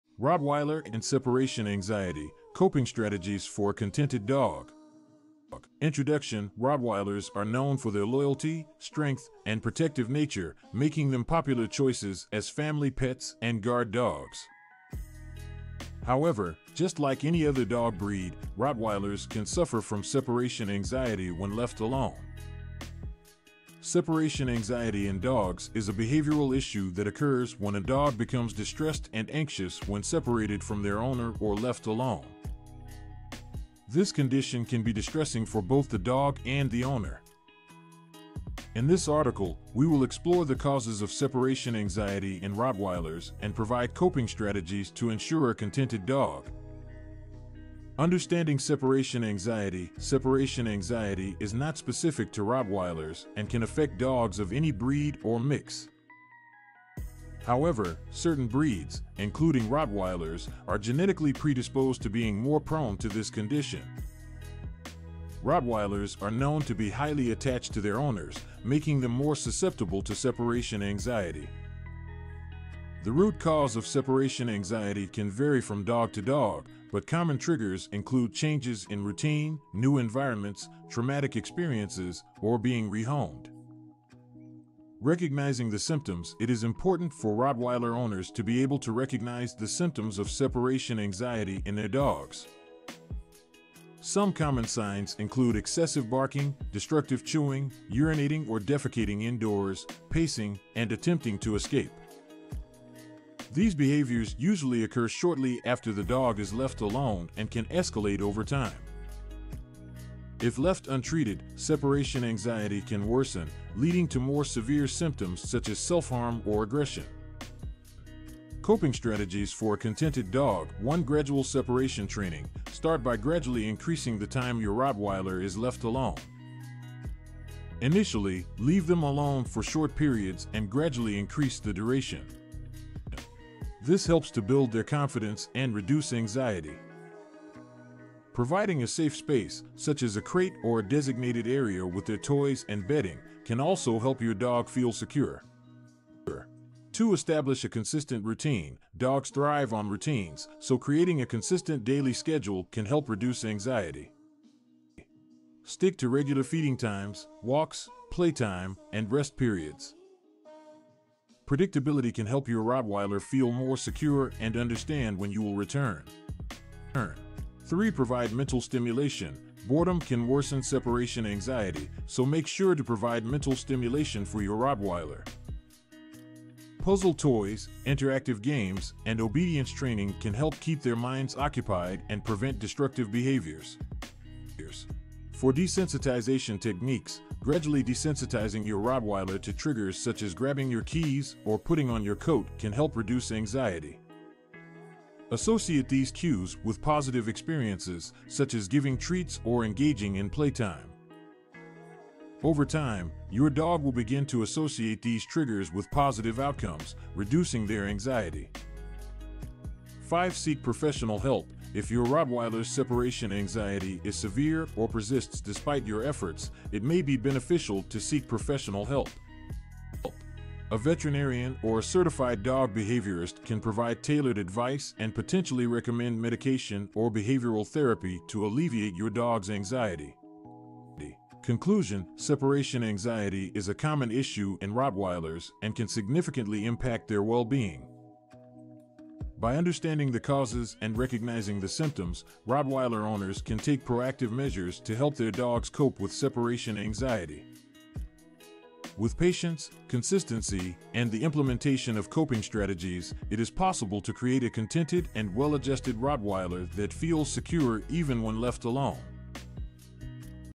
Rottweiler and Separation Anxiety Coping Strategies for a Contented Dog
Rottweilers are known for their loyalty, strength, and protective nature, making them popular choices as family pets and guard dogs  However, just like any other dog breed, Rottweilers can suffer from separation anxiety when left alone  Separation anxiety in dogs is a behavioral issue that occurs when a dog becomes distressed and anxious when separated from their owner or left alone  This condition can be distressing for both the dog and the owner  In this article, we will explore the causes of separation anxiety in Rottweilers and provide coping strategies to ensure a contented dog
00:00 Rottweiler and Separation Anxiety: Coping Strategies for a Contented Dog